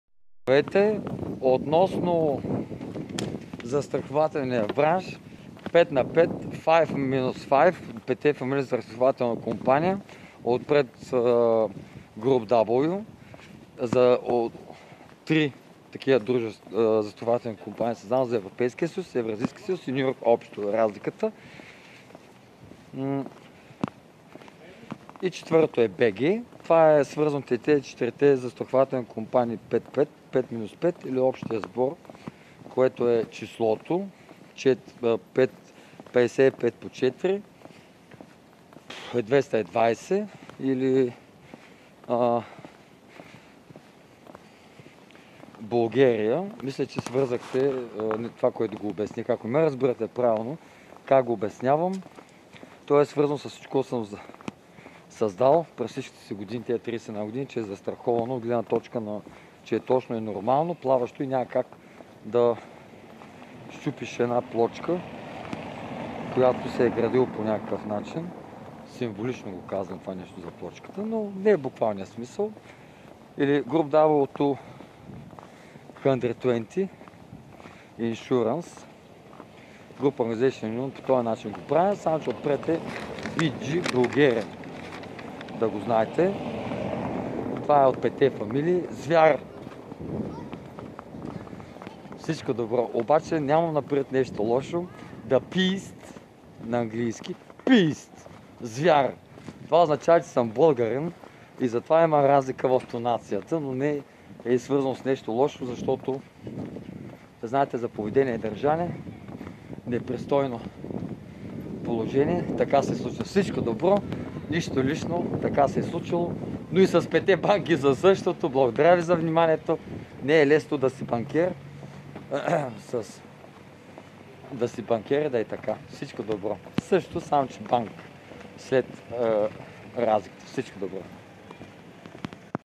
Защо се държа така заради 10г. икономически минуси и работя за себе си за да наваксам сам - за +.
Защо се държа така заради 10 години икономически минуси и работя за себе си за да наваксам сам - /+.